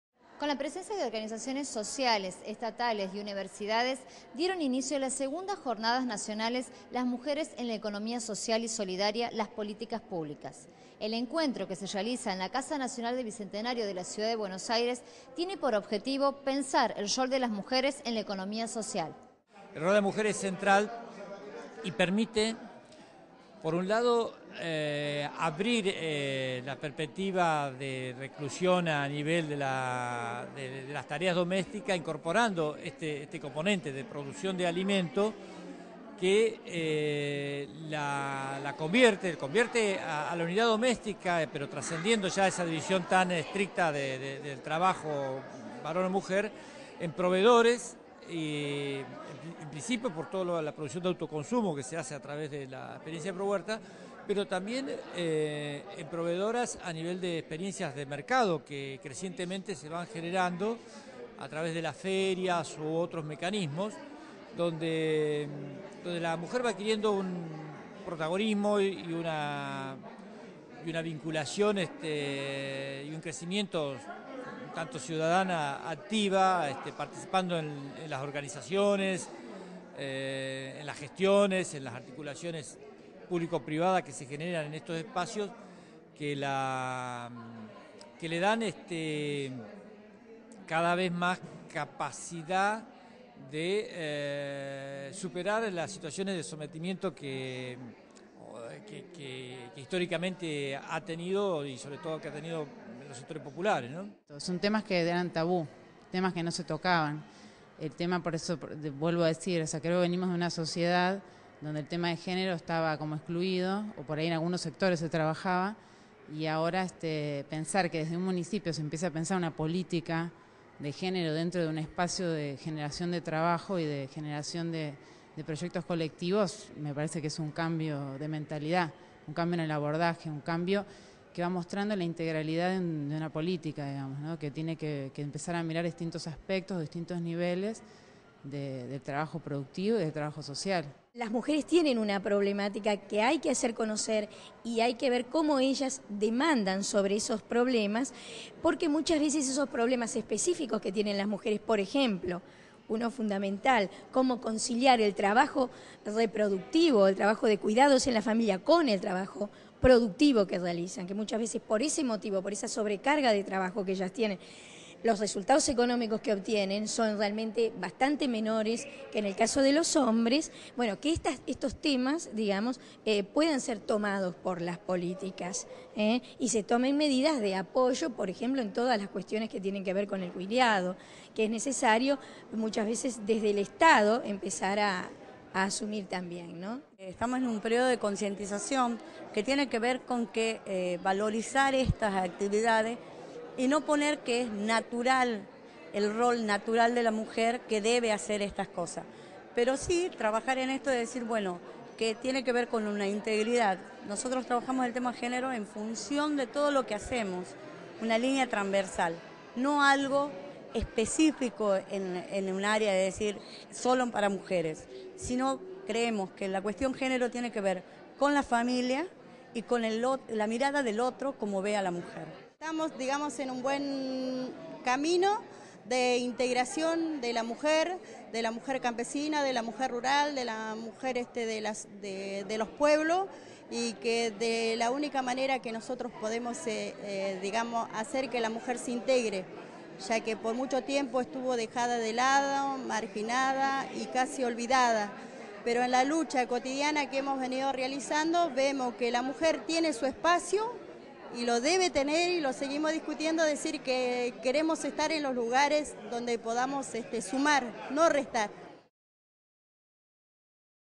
Debatieron sobre el rol de las mujeres en la economía social
En el marco de las   2ª Jornadas Nacionales "Las Mujeres en la Economía Social y Solidaria: las Políticas Públicas" que se realizó en la  Casa Nacional del Bicentenario, el Coordinador Nacional del Programa Pro-Huerta, Roberto Cittadini calificó de "central" el rol de la mujer en la economía social .

El encuentro lo organizaron conjuntamente por la Asociación Lola Mora (ALM), el Programa de Estudios Avanzados en Economía Solidaria del Instituto de Altos Estudios Sociales (IDAES) de la Universidad Nacional de San Martín (UNSAM), el Centro de Estudios Urbanos y Regionales (CEUR) y el Programa ProHuerta de INTA/MINAGRI-MDS y cuentan con el auspicio de la Agencia Española de Cooperación Internacional para el Desarrollo (AECID).